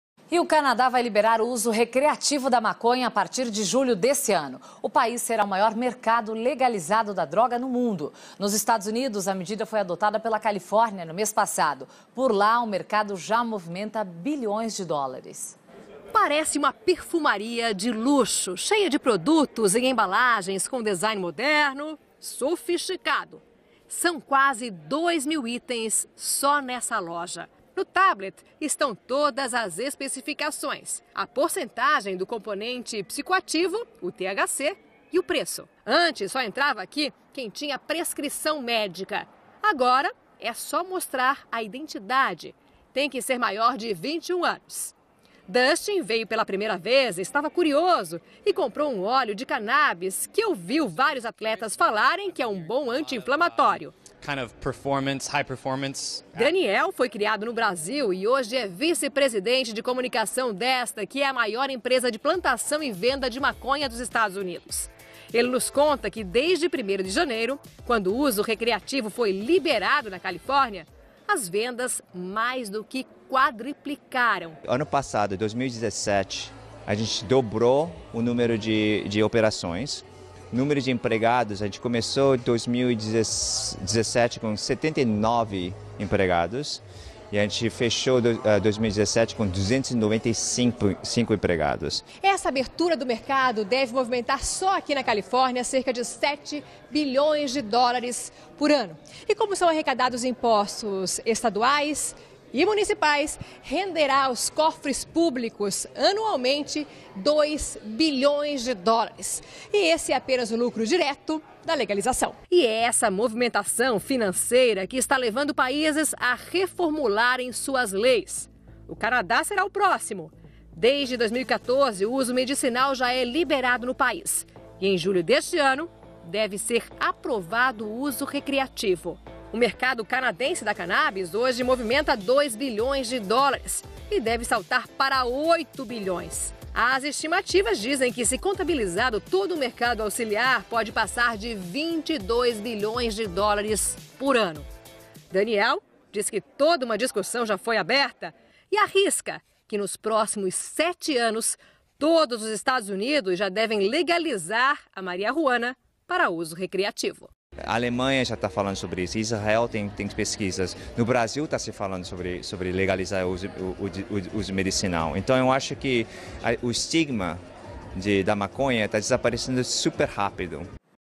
Como está o uso recreativo da maconha na Califórnia e como será no Canadá
A partir de julho deste ano, o Canadá vai liberar o uso recreativo da maconha e deve se tornar o maior mercado legalizado da droga em todo o mundo. O país norte-americano segue a tendência de outras regiões, como o estado da Califórnia, que liberou a substância no primeiro dia de 2018. Desde então, as vendas da maior empresa de plantação e venda de maconha dos Estados Unidos mais do que quadruplicaram. A estimativa é que a legalização movimente mais de 7 bilhões de dólares por ano somente no estado.